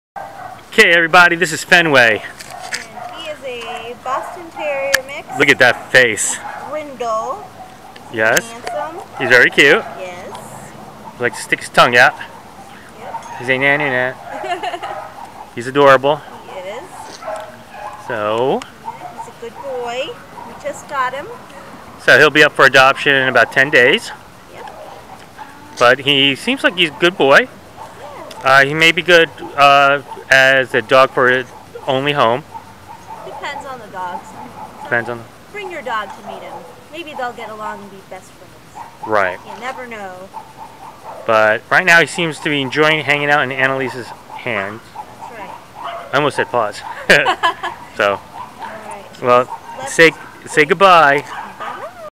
Finway
Meet Finway, a very lovable Boston Terrier looking for a wonderdul home. His estimated date of birth is 11/26/2010.  He has lots of energy but it very friendly. He can be dog selective so if you have a home with a dog the meet and greet will determine if Finway is a perfect fit.  Please consider this precious dog for adoption.

If you are interested in this pet, please fill out an adoption application We will contact you within 48 hours.